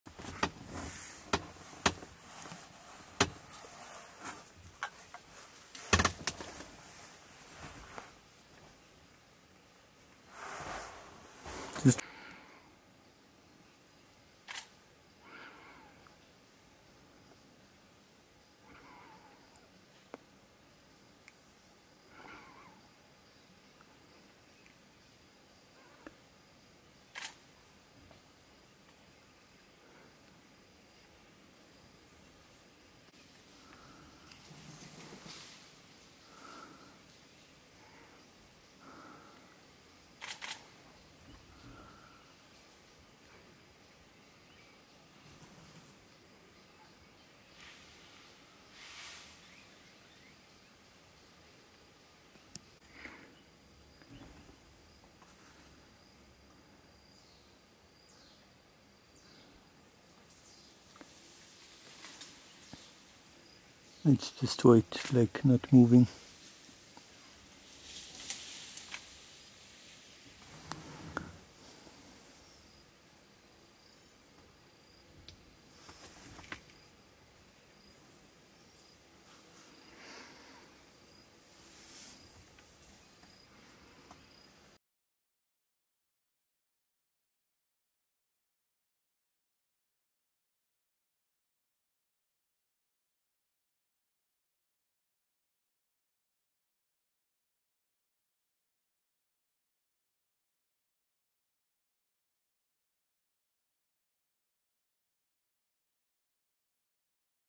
Black and white Colobus ,Guereza / Arusha NP, Tanzania
The black-and-white colobus monkey, native to Africa, is a striking creature adorned in contrasting hues. Its glossy black fur starkly contrasts with its snowy white mantle cascading down its shoulders, creating a stunning visual allure. These agile primates, with their long tails and expressive faces framed by a distinguished white fringe of hair, navigate treetops with grace. Their unique appearance serves a purpose beyond aesthetics; it aids in camouflage amidst the dense foliage. Known for their arboreal lifestyle and captivating appearance, these colobus monkeys embody a natural elegance in their black and white attire, a testament to the beauty found in wildlife diversity.